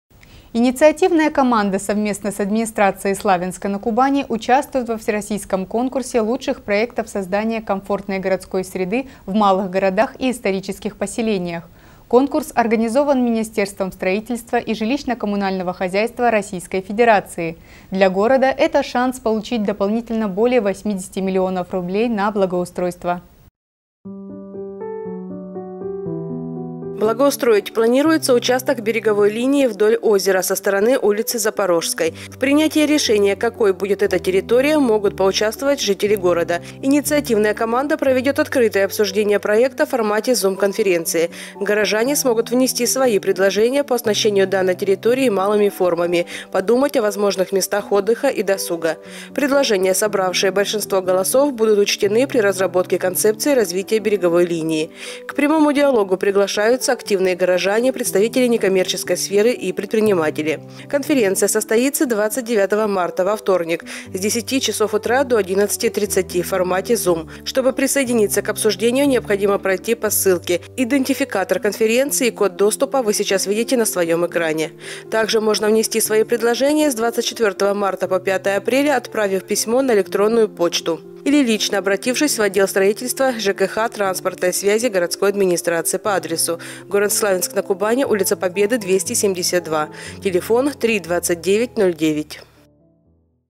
Благоустройство города
Инициативная команда совместно с администрацией Славянска-на-Кубани участвует во Всероссийском конкурсе лучших проектов создания комфортной городской среды в малых городах и исторических поселениях.
Конкурс организован Министерством строительства и жилищно-коммунального хозяйства Российской Федерации. Для города - это шанс получить дополнительно более 80 миллионов рублей на благоустройство